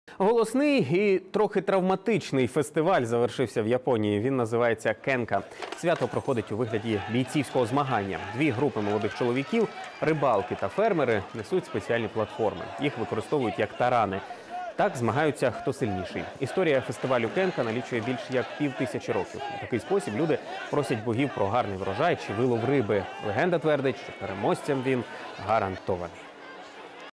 Голосний і трохи травматичний фестиваль завершився в Японії
Він називається Кенка. Свято проходить у вигляді бійцівського змагання. Дві групи молодих чоловіків – рибалки та фермери – несуть спеціальні платформи. Їх використовують як тарани. Так змагаються, хто сильніший. Історія фестивалю Кенка налічує більше як півтисячі років. У такий спосіб люди просять богів про гарний врожай чи вилов риби. Легенда твердить, що переможцям він гарантований.